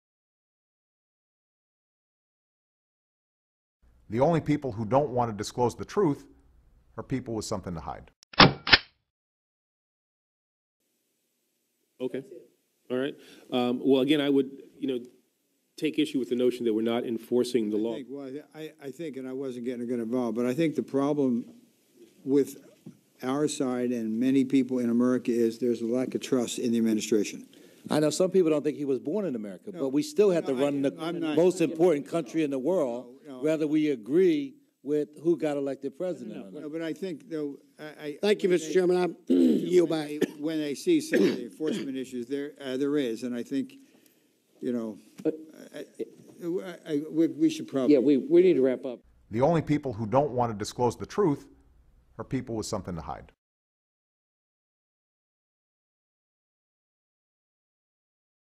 Dem. Rep. At Committee Hearing: People Don't Think Obama Born In America
Dem. Rep. Birther Jab At Committee Hearing;People Don't Think Obama Born In America - 4/4/2014 - MORE